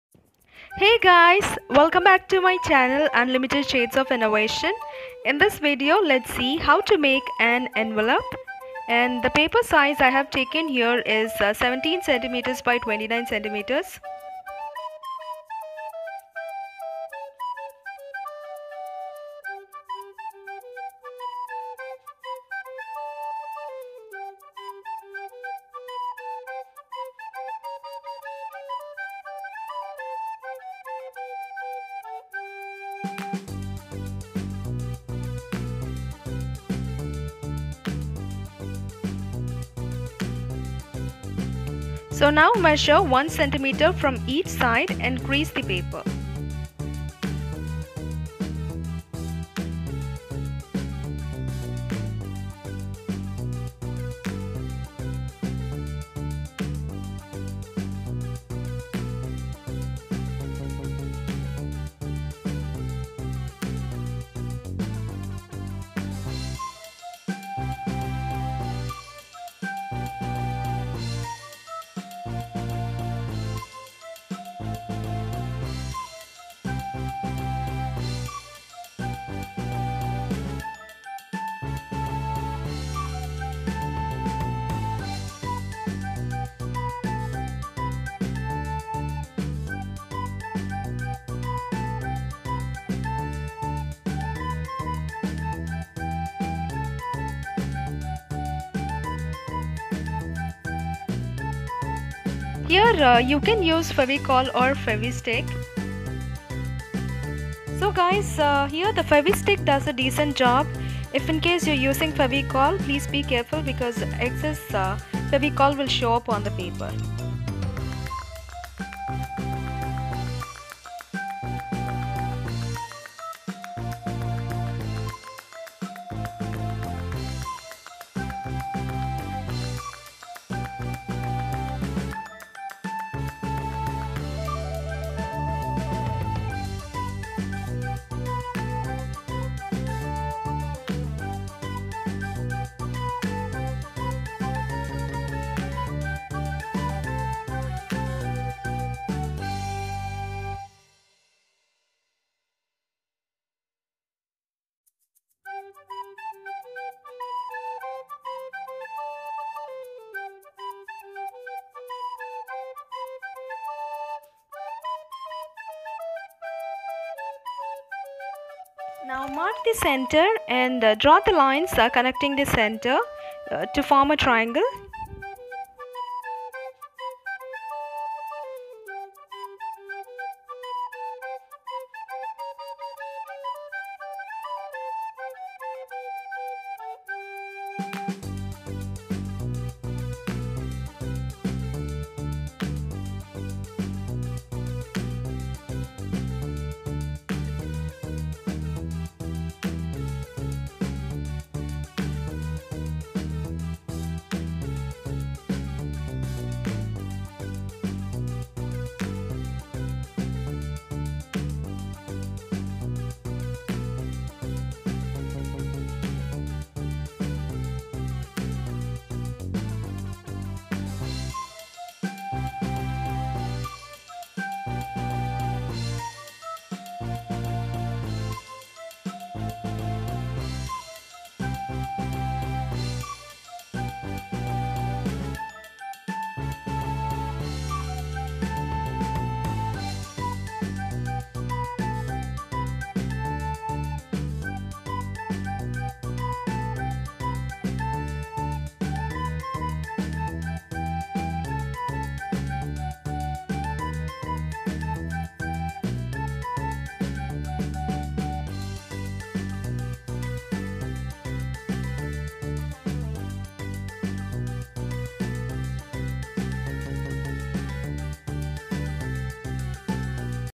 How to make an Envelope/DIY Simple Envelope/Handmade Envelope for Greeting Card/DIY Envelope/USI–65
UNLIMITED SHADES OF INNOVATION - 'DO SOMETHING CREATIVE EVERYDAY'

Art & Craft by K. Sushma Eunice (MTech - IT).

How to make an Envelope/DIY Simple Envelope/Handmade Envelope for Greeting Card/Envelope Making/USI–65

Material Required:
1. Colour Papers
2. Craft Scissors
3. Fevicol/Fevistick
4. Scale
5. Scoring Tool/Pen without Ink(DIY Scoring Tool)

‘EXPLORE THE CREATIVE WORLD'